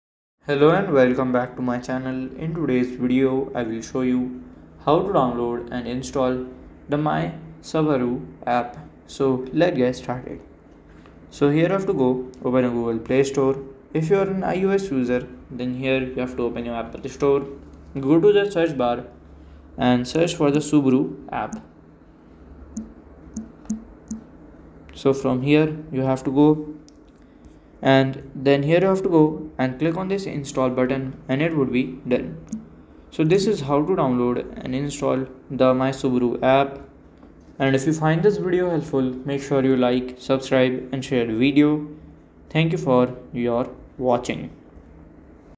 ✅ How To Download and Install My Subaru App (Full Guide)
Enhance your Subaru driving experience with the My Subaru App! This comprehensive guide provides step-by-step instructions for downloading and installing the app on your device. From scheduling service appointments to accessing vehicle health reports, My Subaru offers convenience and connectivity for Subaru owners.

Key Learning Points:

Step-by-step instructions for downloading and installing the My Subaru App
Tips for scheduling maintenance, tracking vehicle performance, and accessing roadside assistance effectively
Overview of key features and Subaru owner benefits